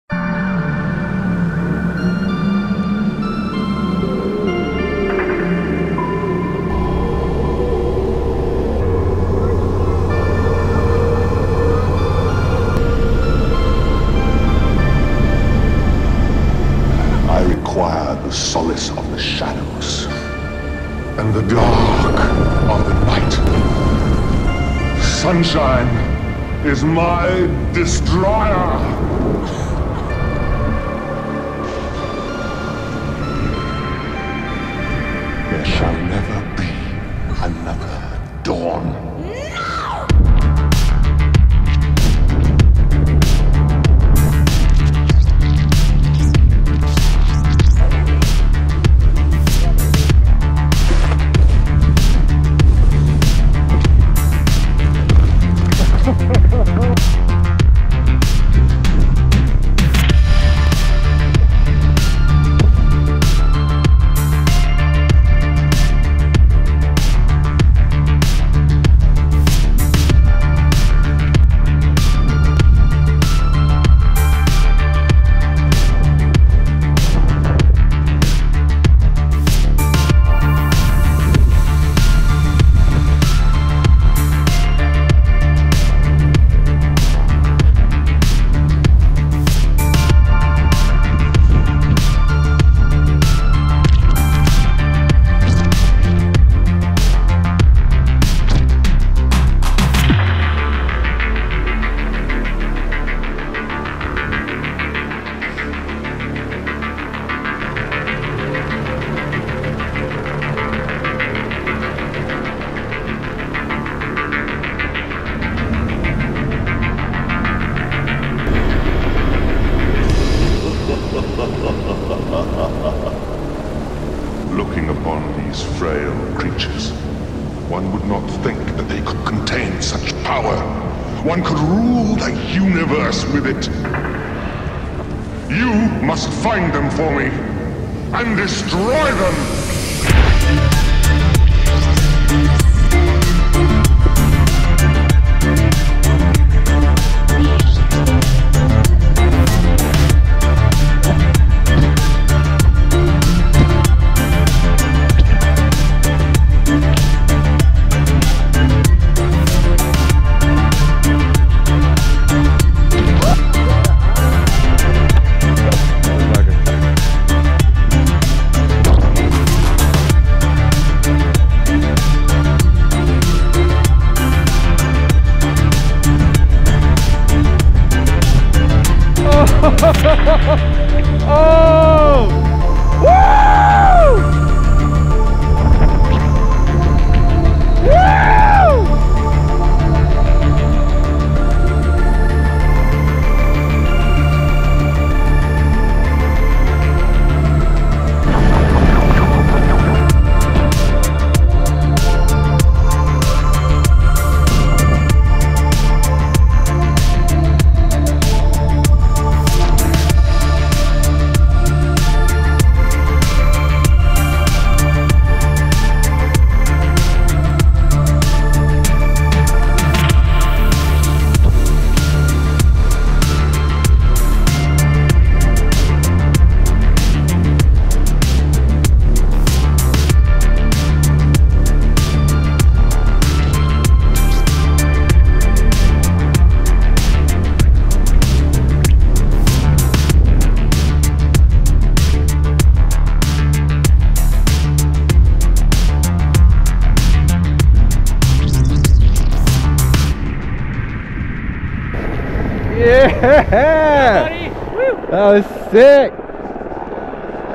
February 2020 @ Ripley Creek
Thanks to Brandon Meier for this awesome heliski edit from February 2020.